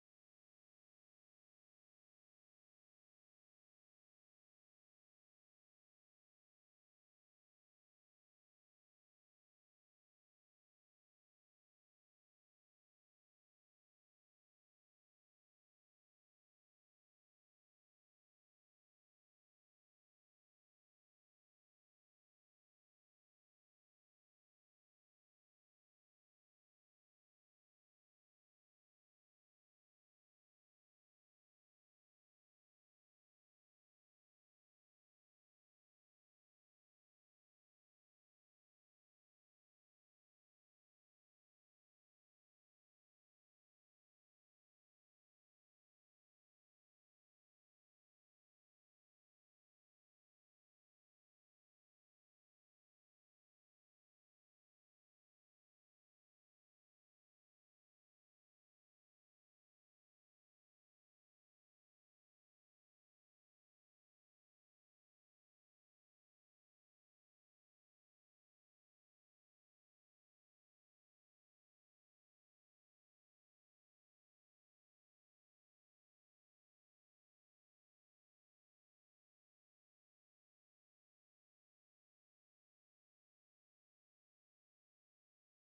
BenQ ZOWIE RL2460S：パススルー出力
4Gamer.netの記事，

BenQ ZOWIE「RL2460S」レビュー。新しくなった「格闘ゲーマー向けディスプレイ」は今回もゲーム機ユーザーの鉄板か

で参照しているムービーです。詳細は記事をチェックしてください。